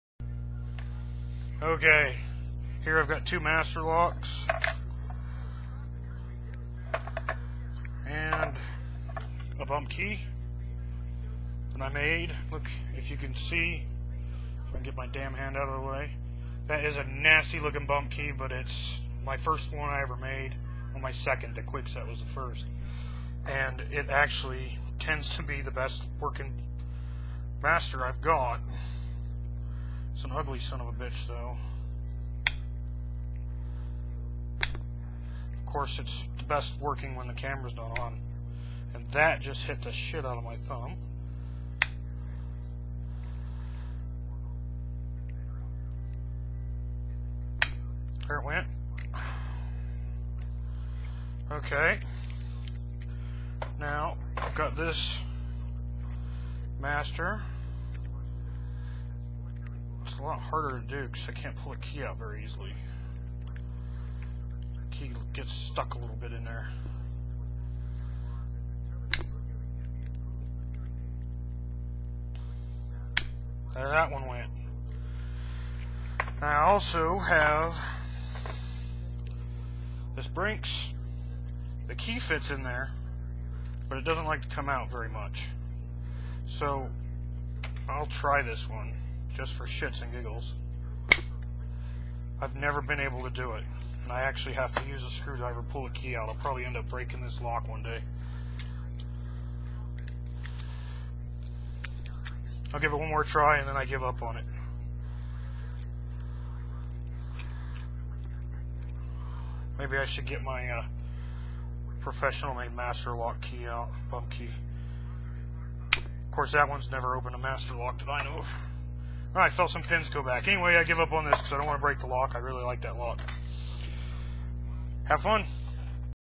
Bumping 2 master padlocks with an ugly-ass bump key
One of the first two bump keys I made, ugly but it works.